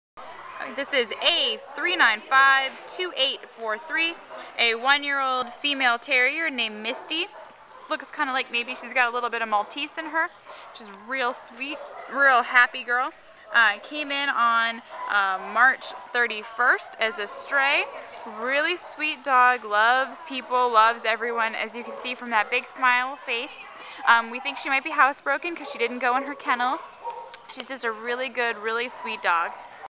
A3952843 - Misty
Misty is a one year old female Maltese mix who is so cute that she was adopted within moments of having her photo taken!  She came into the shelter as a stray on March 31st, and likes nothing more than being held and giving kisses.  It was no surprise that she was adopted so fast!